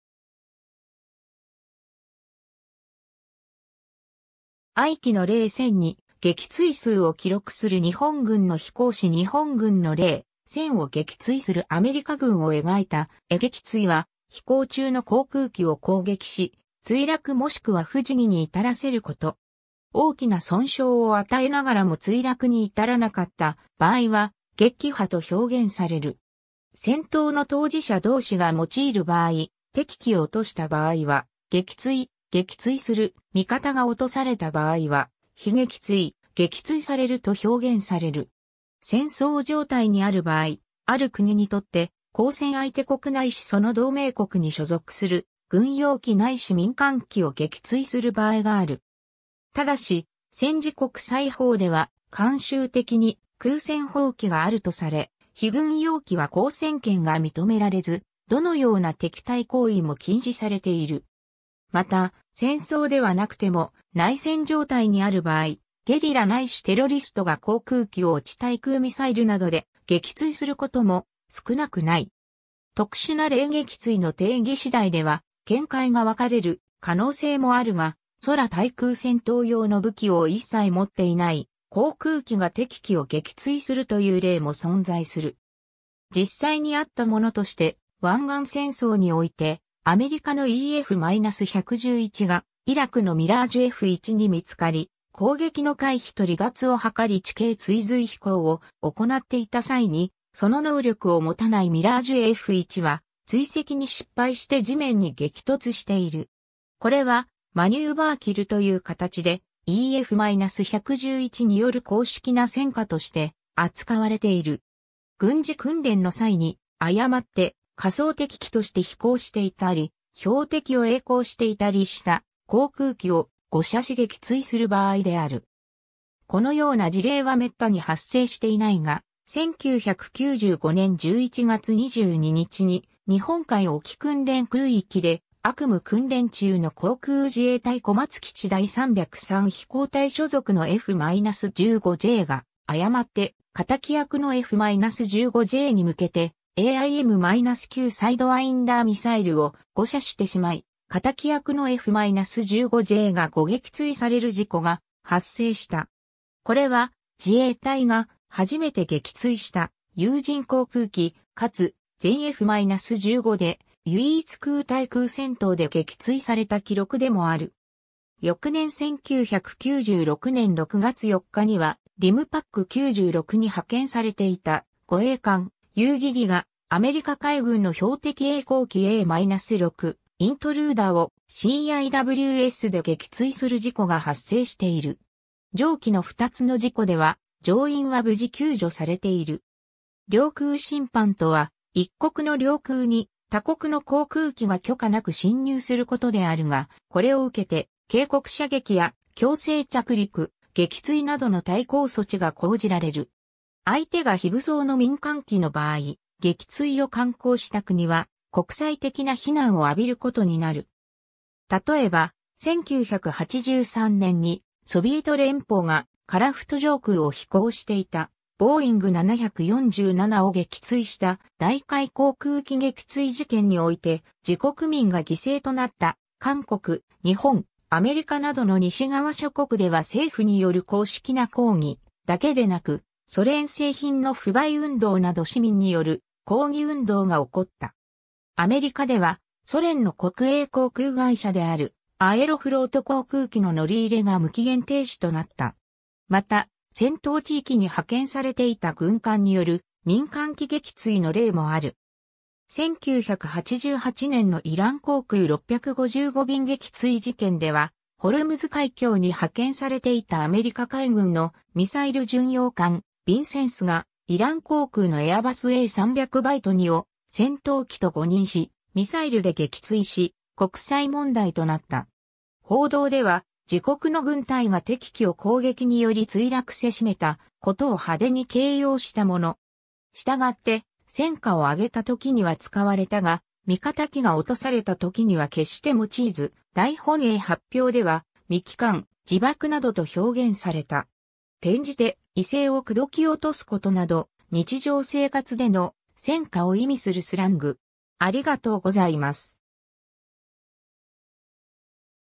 撃墜
愛機の零戦に、撃墜数を記録する日本軍の飛行士 日本軍の零戦を撃墜するアメリカ軍を描いた絵 撃墜（げきつい）は、飛行中の航空機を攻撃し、墜落もしくは不時着に至らせること。
大きな損傷を与えながらも墜落に至らなかった場合は「撃破」と表現される。
戦闘の当事者同士が用いる場合、敵機を落とした場合は「撃墜」「撃墜する（した）」、味方が落とされた場合は「被撃墜」「撃墜される（された）」と表現される。
 戦争状態にある場合、ある国にとって交戦相手国ないしその同盟国に所属する軍用機ないし民間機を撃墜する場合がある。
ただし、戦時国際法では慣習的に空戦法規があるとされ、非軍用機は交戦権が認められず、どのような敵対行為も禁止されている。
また、戦争ではなくても内戦状態にある場合、ゲリラないしテロリストが航空機を地対空ミサイルなどで撃墜することも少なくない。
 特殊な例 撃墜の定義次第では見解が分かれる可能性もあるが、空対空戦闘用の武器を一切持っていない航空機が敵機を撃墜するという例も存在する。
実際にあったものとして、湾岸戦争においてアメリカのEF-111がイラクのミラージュF1に見つかり、攻撃の回避と離脱を図り地形追随飛行を行なっていた際に、その能力を持たないミラージュF1は追跡に失敗して地面に激突している。
これはマニューバーキルという形で、EF-111による公式な戦果として扱われている。
 軍事訓練の際に、誤って仮想敵機として飛行していたり、標的を曳航していたりした航空機を誤射し撃墜する場合である。
 このような事例は滅多に発生していないが、1995年11月22日に日本海沖訓練空域でACM訓練中の航空自衛隊小松基地第303飛行隊所属のF-15Jが、誤って敵役のF-15Jに向けてAIM-9 サイドワインダーミサイルを誤射してしまい、敵役のF-15Jが誤撃墜される事故が発生した。
これは、自衛隊が初めて撃墜した有人航空機、かつ、全F-15で唯一空対空戦闘で撃墜された記録でもある。
 翌年1996年6月4日には、リムパック96に派遣されていた護衛艦「ゆうぎり」が、アメリカ海軍の標的曳航機A-6 イントルーダーをCIWSで撃墜する事故が発生している。
 上記の2つの事故では乗員は無事救助されている。
 領空侵犯とは、一国の領空に他国の航空機が許可なく侵入することであるが、これを受けて警告射撃や強制着陸、撃墜などの対抗措置が講じられる。
相手が非武装の民間機の場合、撃墜を敢行した国は国際的な非難を浴びることになる。
 たとえば、1983年にソビエト連邦が樺太（サハリン）上空を飛行していたボーイング747を撃墜した大韓航空機撃墜事件において、自国民が犠牲となった韓国・日本・アメリカなどの西側諸国では政府による公式な抗議だけでなく、ソ連製品の不買運動など市民による抗議運動が起こった。
アメリカではソ連の国営航空会社であるアエロフロート航空機の乗り入れが無期限停止となった。
 また、戦闘地域に派遣されていた軍艦による民間機撃墜の例もある。
1988年のイラン航空655便撃墜事件では、ホルムズ海峡に派遣されていたアメリカ海軍のミサイル巡洋艦「ヴィンセンス」が、イラン航空のエアバスA300B2を戦闘機と誤認しミサイルで撃墜し国際問題となった。
 報道では、自国の軍隊が敵機を攻撃により墜落せしめたことを派手に形容したもの。
従って戦果を挙げた時には使われたが、味方機が落とされた時には決して用いず、大本営発表では「未帰還」（実際は体当たりを目論んだのではなく、単に撃墜された場合でも）「自爆」などと表現された。
 転じて異性を口説き落とすことなど、日常生活での「戦果」を意味するスラング。